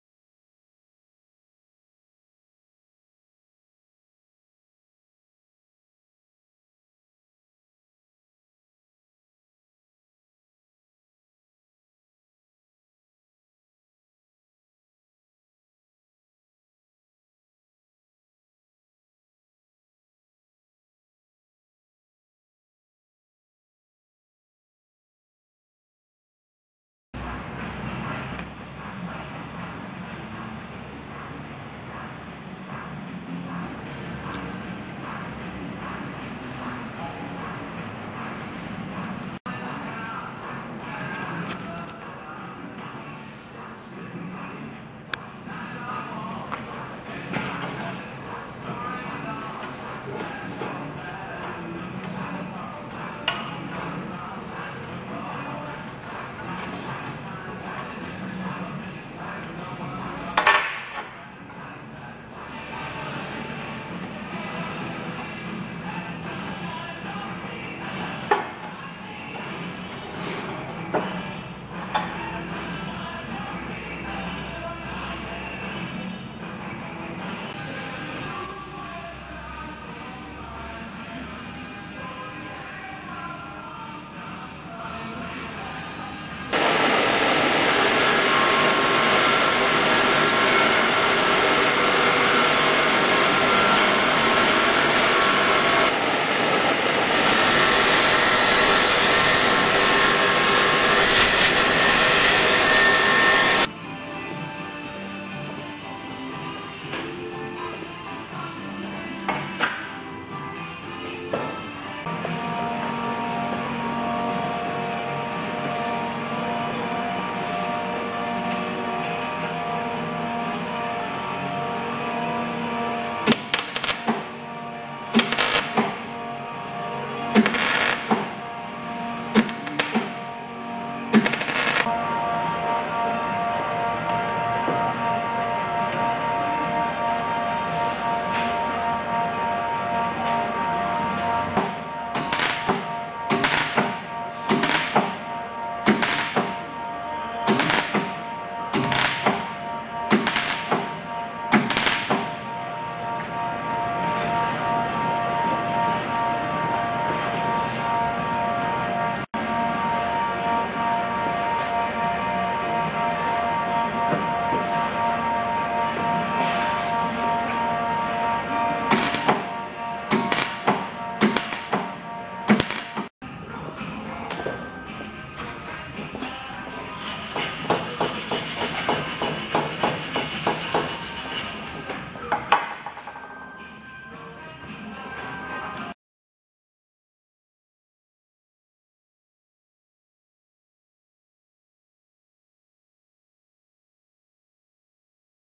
Garden Furniture
How to repair some crappy garden furniture that's broken!